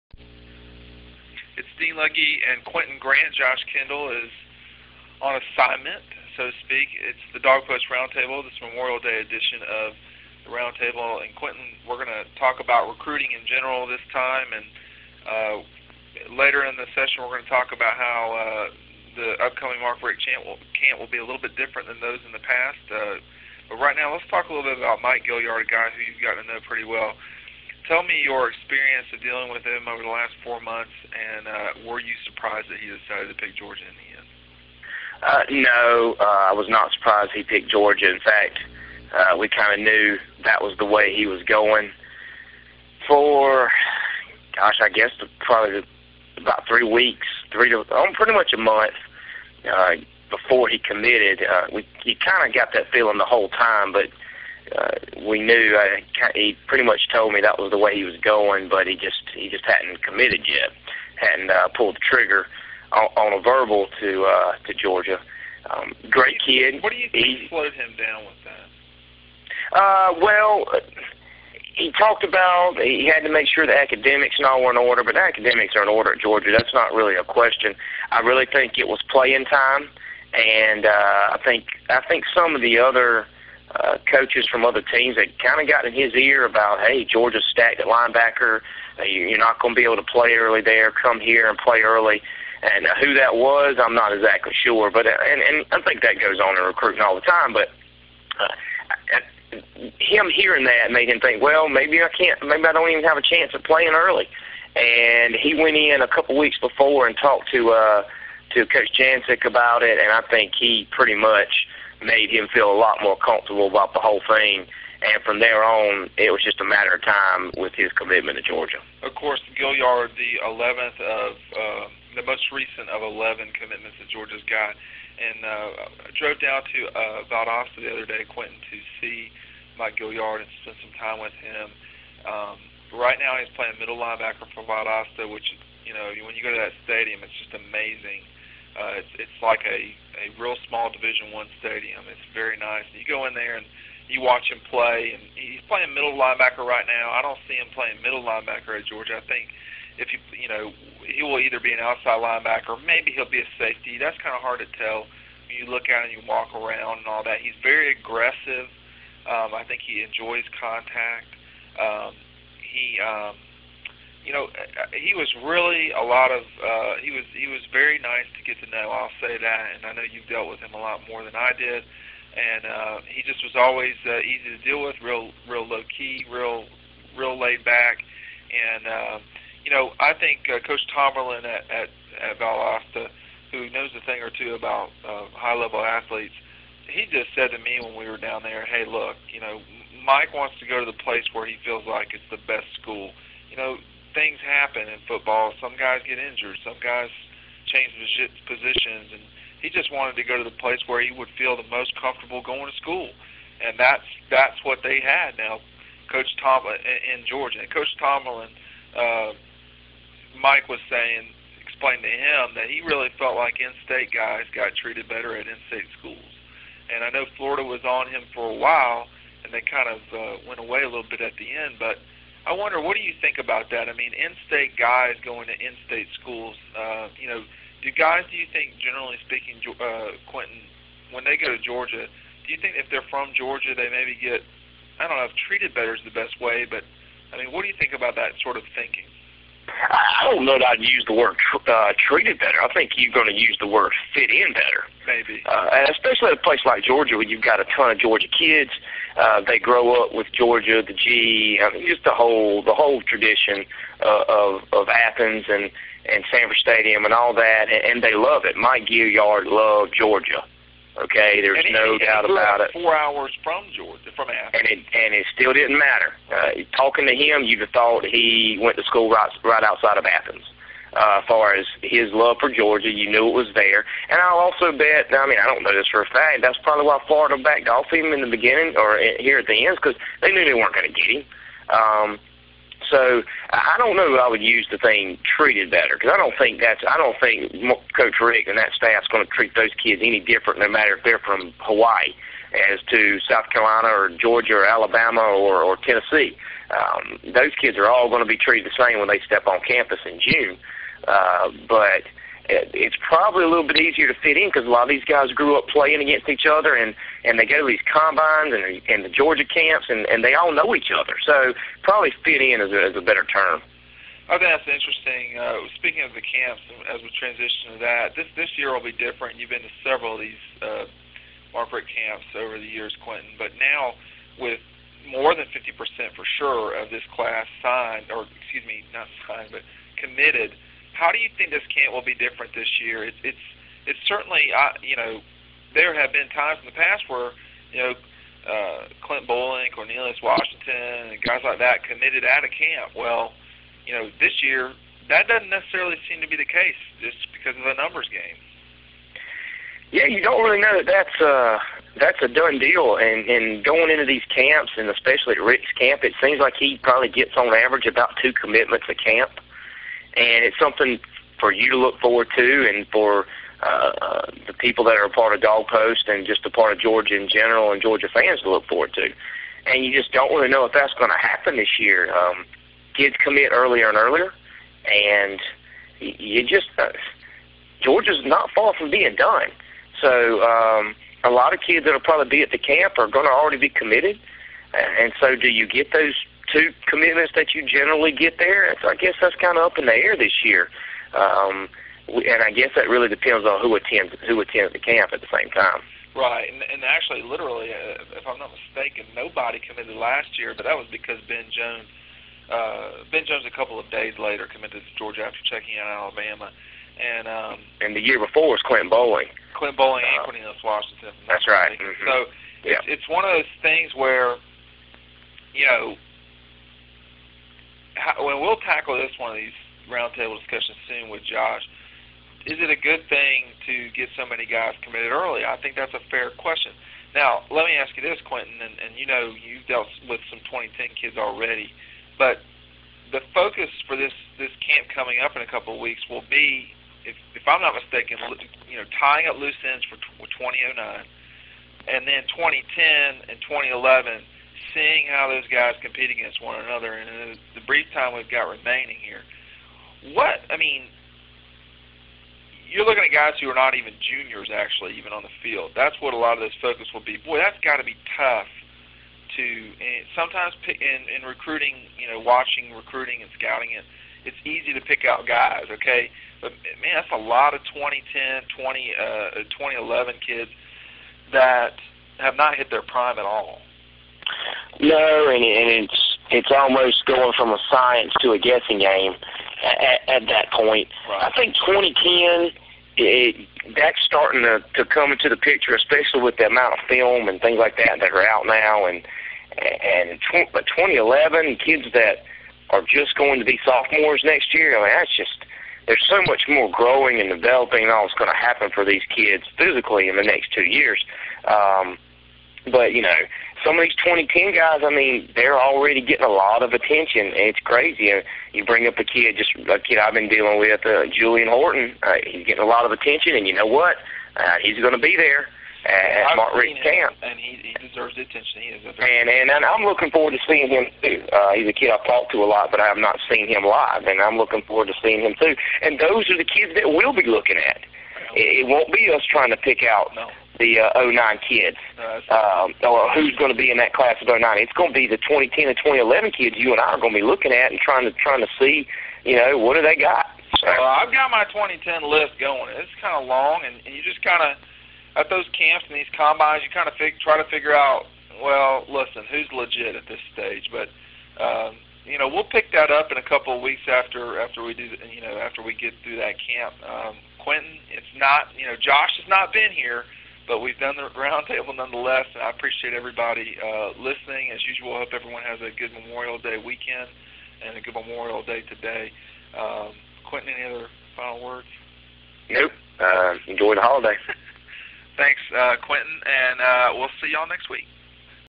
May 26, 2008: DawgPost Roundtable
ATHENS - Listen to Dean Legge, Josh Kendall and Quentin Grant talk about Georgia football.